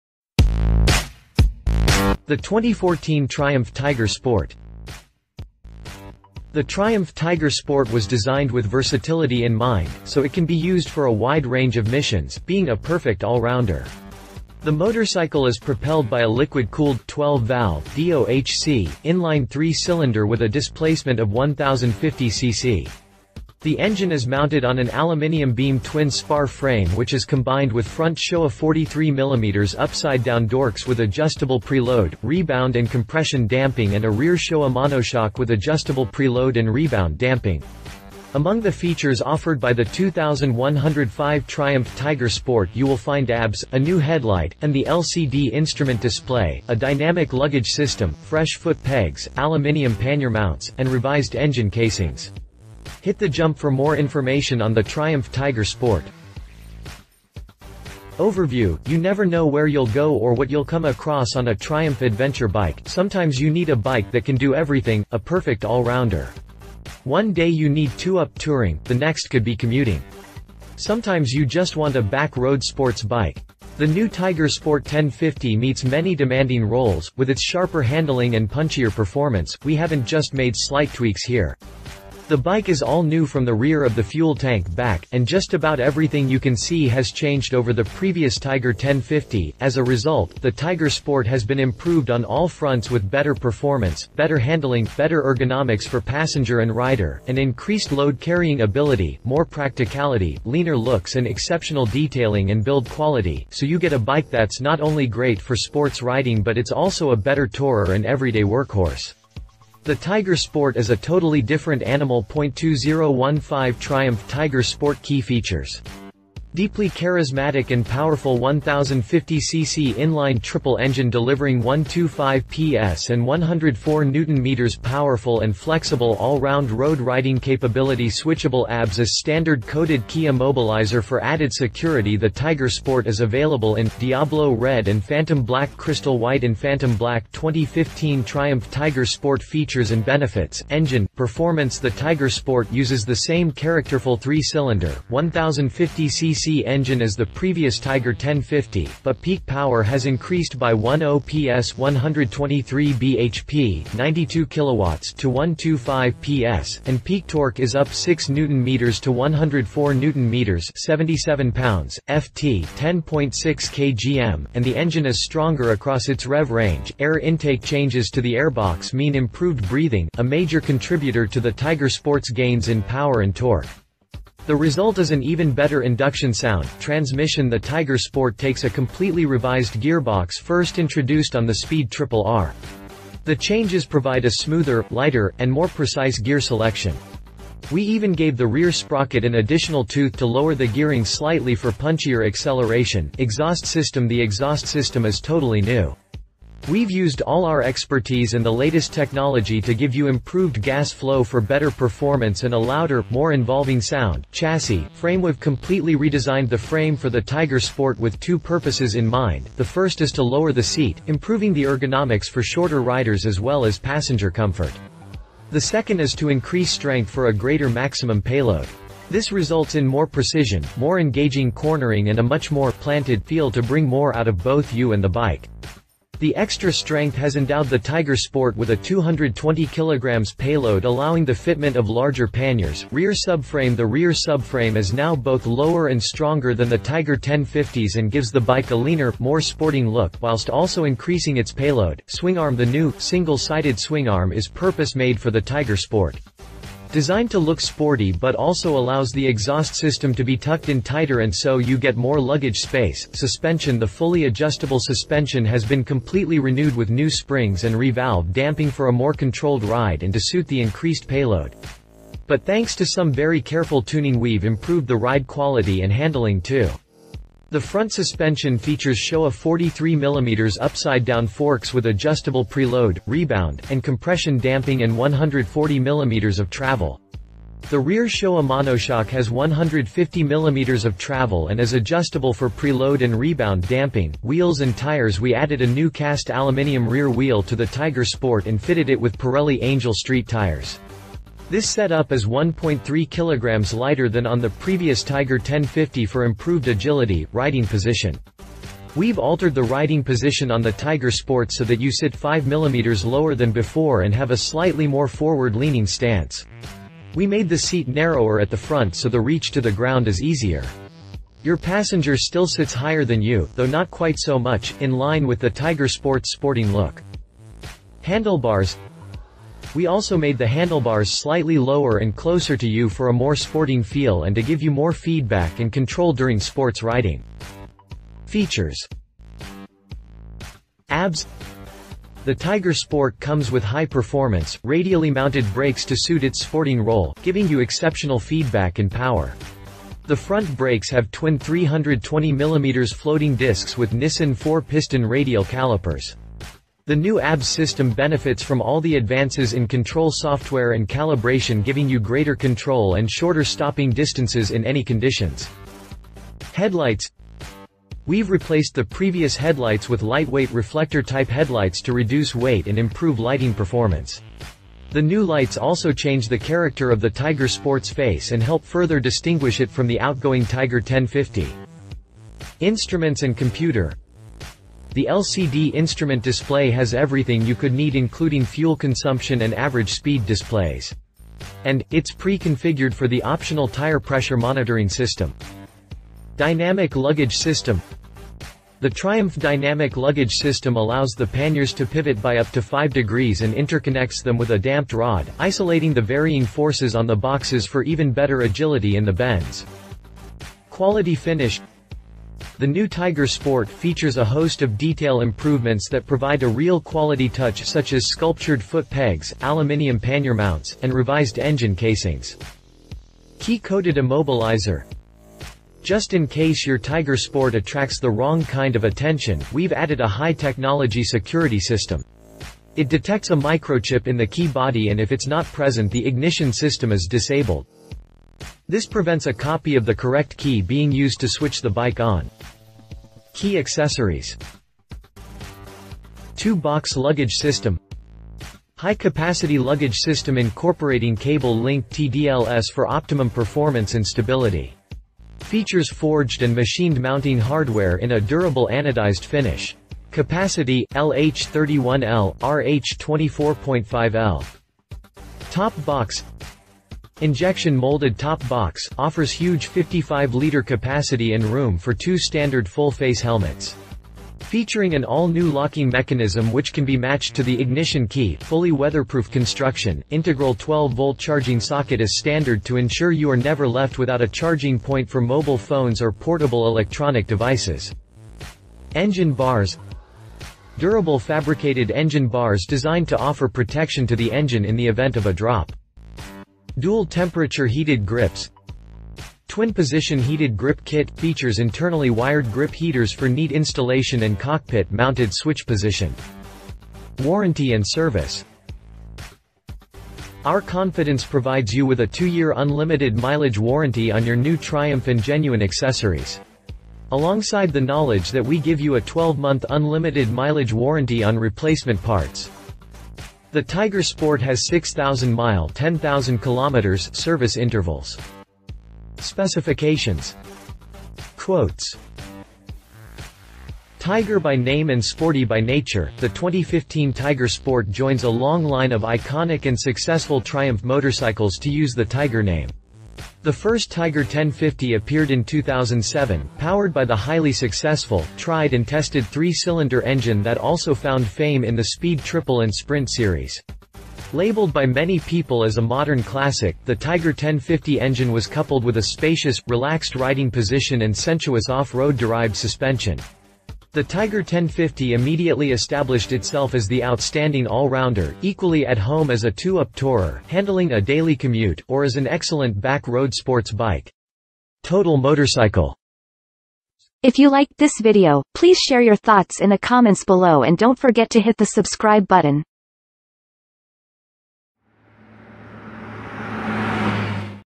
2014 Triumph Tiger Sport
The triumph Tiger Sport was designed with versatility in mind, so it can be used for a wide range of missions, being a perfect all-rounder.
The motorcycle is propelled by a liquid-cooled, 12 valve, DOHC, in-line 3-cylinder with a displacement of 1050 cc. The engine is mounted on an aluminium beam twin-spar frame which is combined with front Showa 43 mm upside down dorks with adjustable preload, rebound and compression damping and a rear showa monoshock with adjustable preload and rebound damping.
Among the features offered by the 2105 Triumph Tiger Sport you will find ABS, a new headlight, an The LCD instrument display, a dynamic luggage system, fresh foot pegs, aluminium pannier mounts, and revised engine casings.
Hit the jump for more information on the Triumph Tiger Sport.